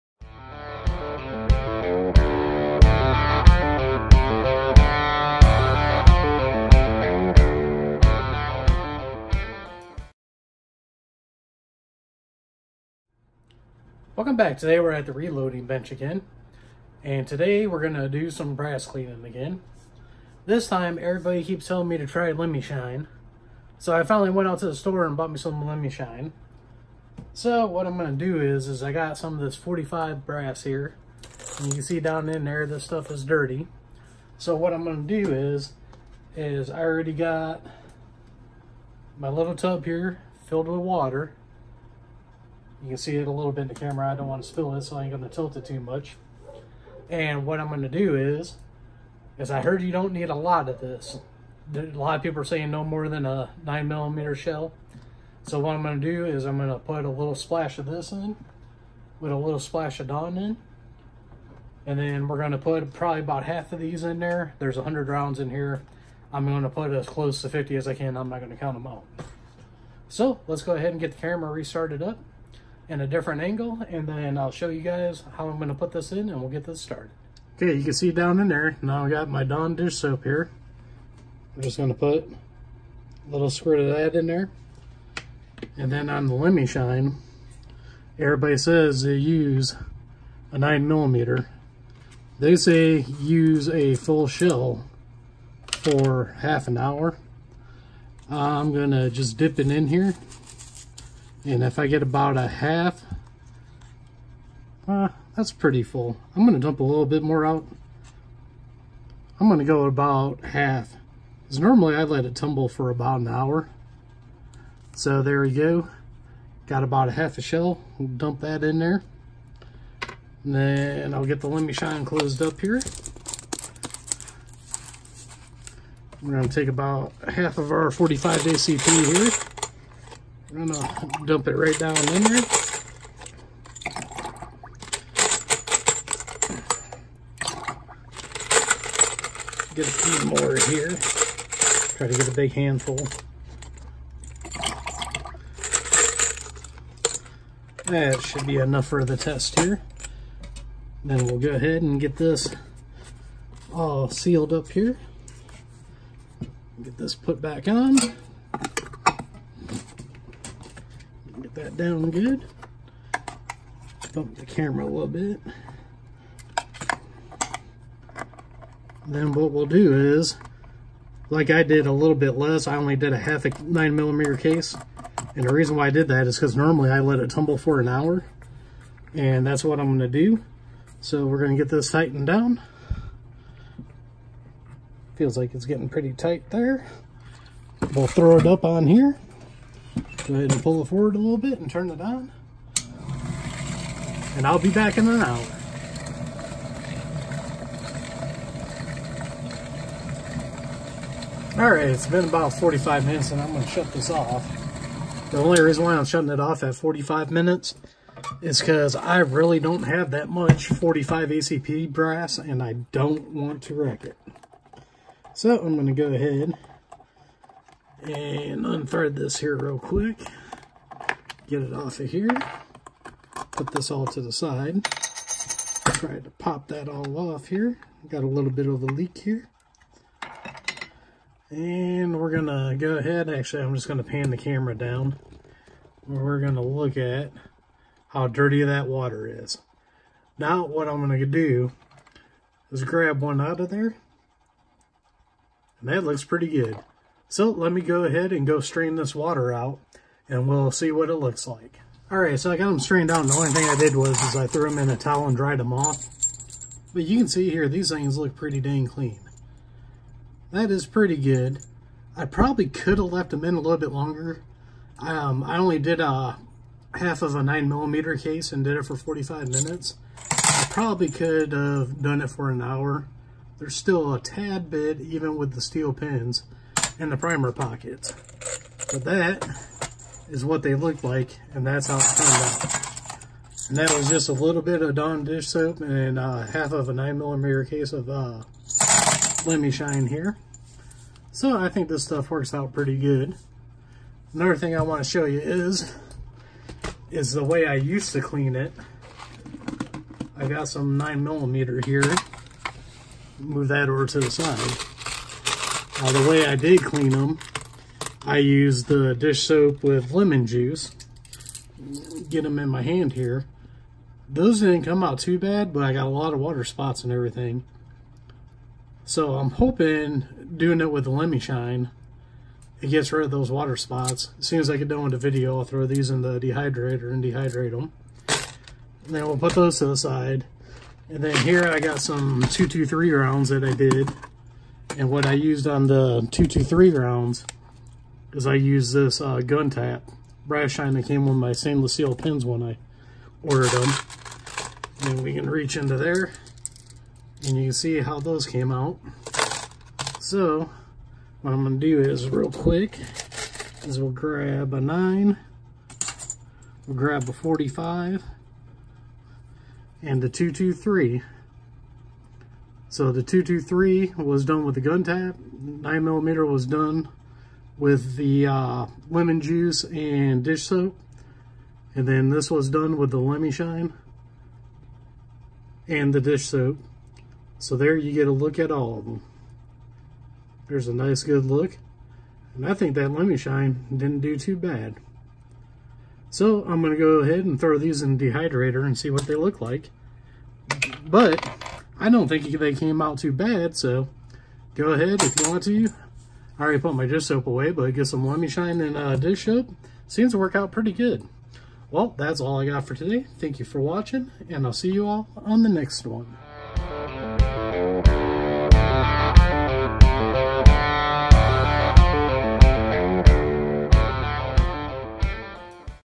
Cleaning brass with Lemi Shine & Dish Soap
Trying out Lemi Shine to clean brass. A lot of people recommend trying it.

Amazon Storefront